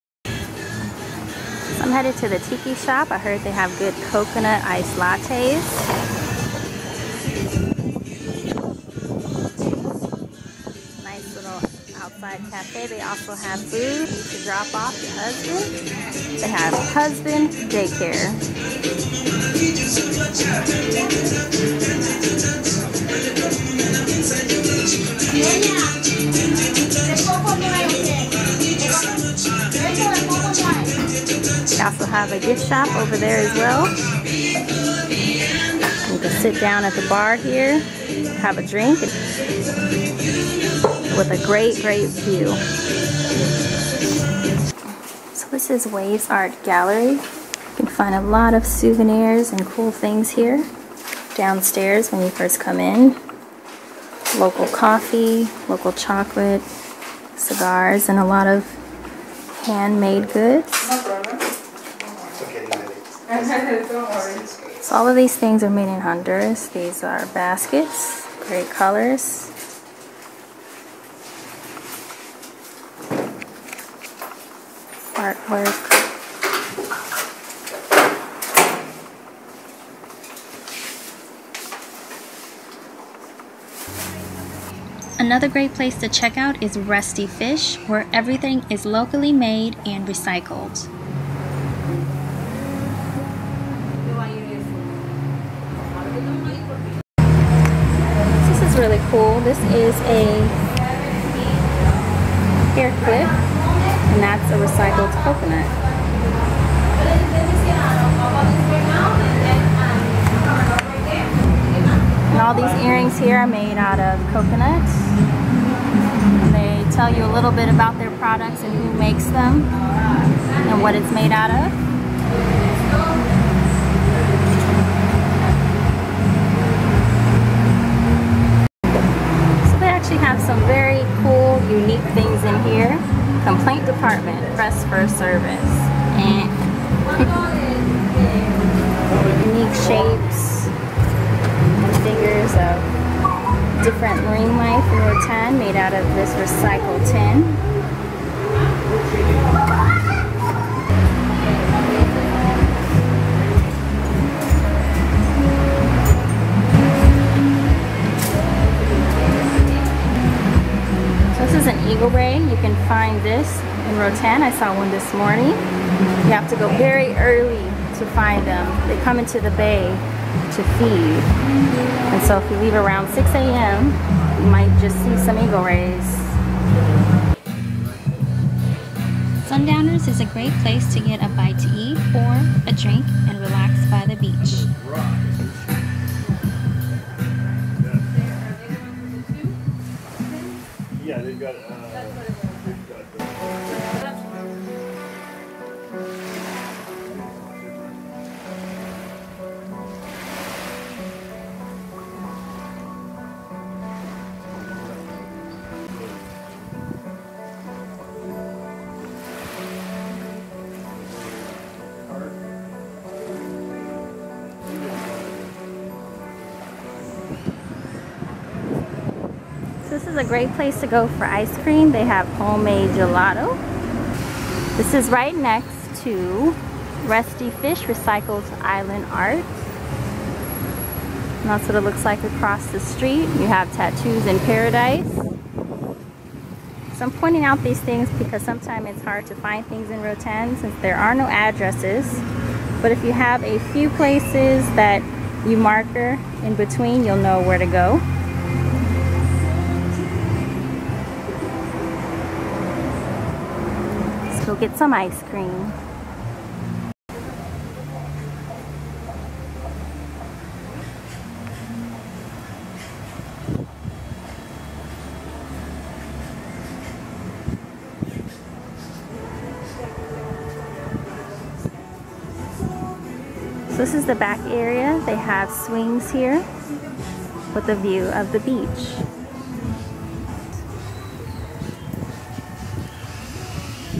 Roatan West End Video Tour - Shopping & Things to see in Roatan West End
Travel Roatan - Video tour of Roatan West End to show you some of the shops and places to see in Roatan West End. Plus, find out where to buy the best souvenirs. Just one of many videos showing you what you can expect to see in Roatan West End.

Places visited in the video:
Sundowners Bar and Restaurant
Rusty Fish  (Recycled art and souvenirs)
Waves of Art
Tiki Bar